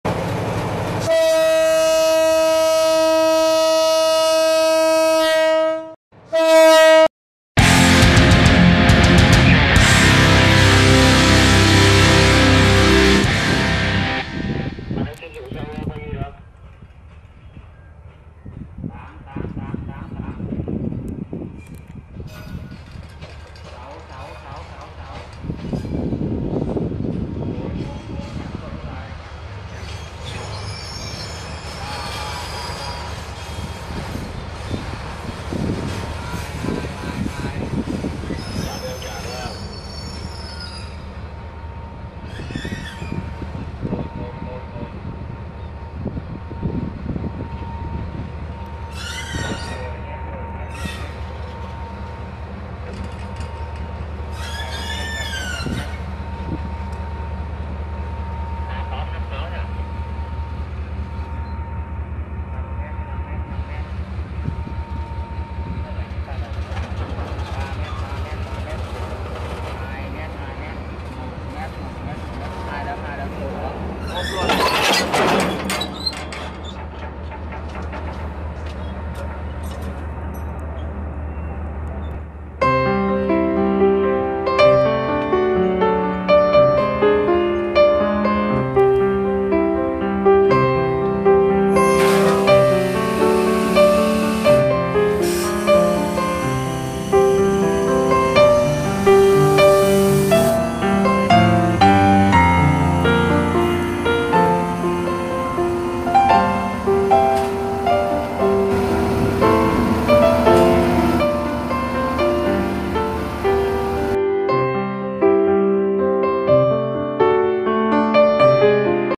CẬN CẢNH ĐẦU MÁY XE LỬA ĐẨY XE DẦU NỐI VÀO TOA TÀU HÀNG SY1 (04/02/2018)
Đường Sắt Việt Nam - Ga Trảng Bom: Đầu máy xe lửa D19E-934 đầy xe dầu nối vào toa tàu hàng SY1 chiều ngày 04/02/2018.
04022018